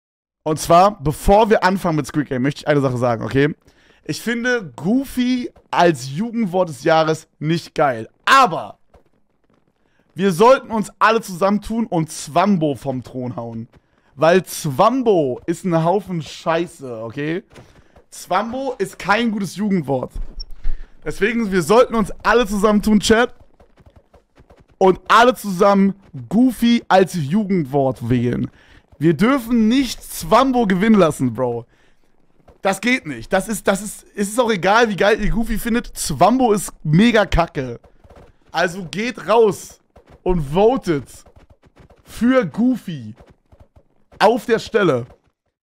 Papaplatte schlägt das Jugendwort 2023 vor
Papaplatte Clips Livestream Highlights